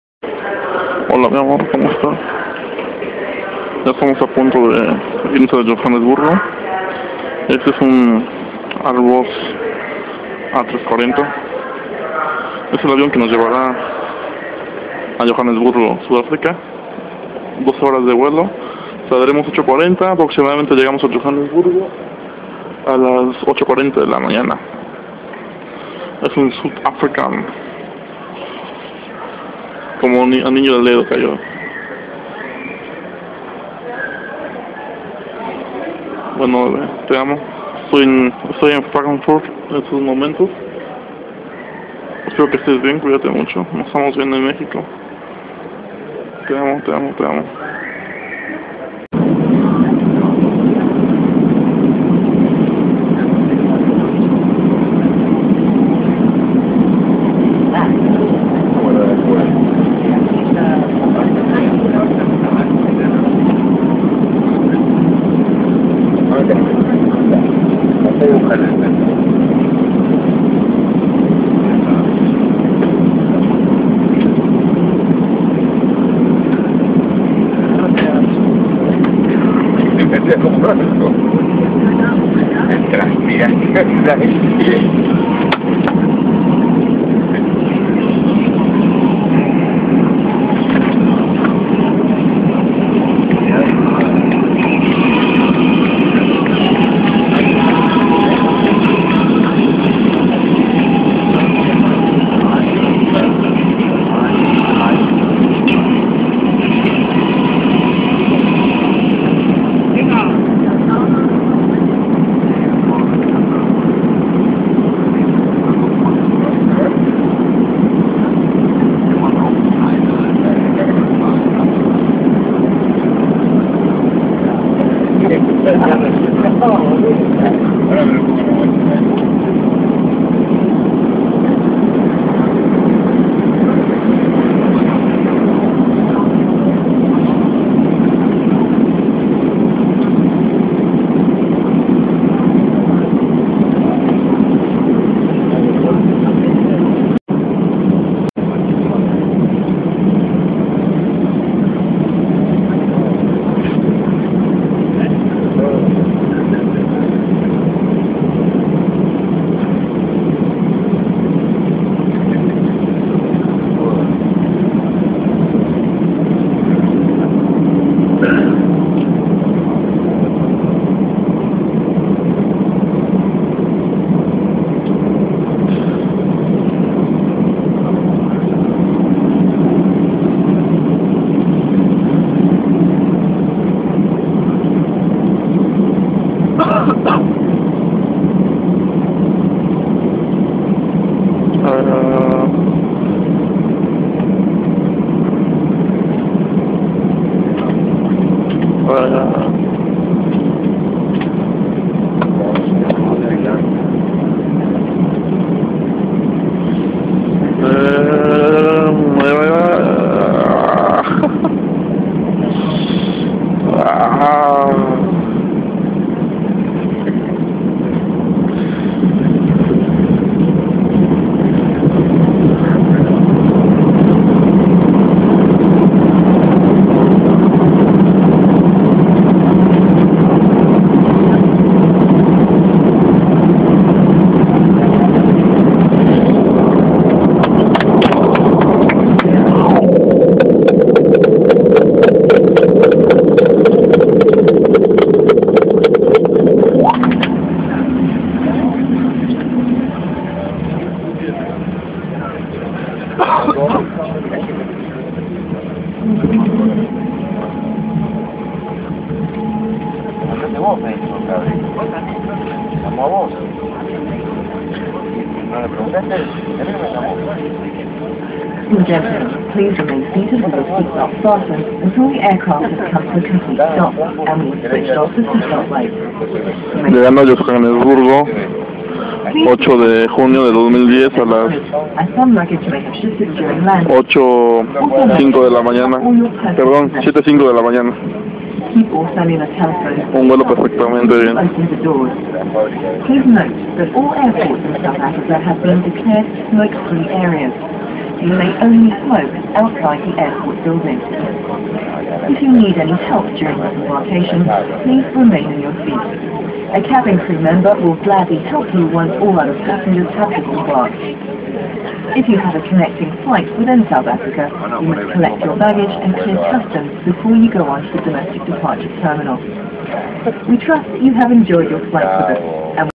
Airbus A340-600 South African Airways De Frankurt A Johannsburg SouthAfrica
08 de junio del 2010 el Airbus A340-600 de South African Airways esta apunto de aterrizar en el Aeropuerto de Johannsburgo en Sudafrica, procedentes de Frankfurt Alemania